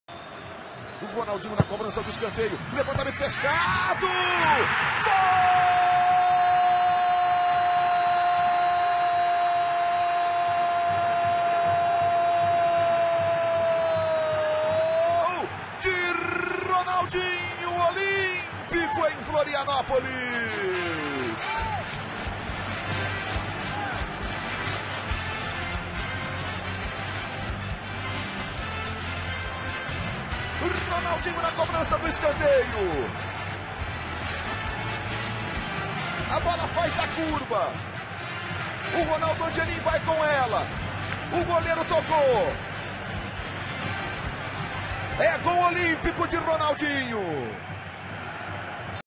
Ronaldinho Scores Direct from Corner
Brazilian legend clearly loving it at Flamengo!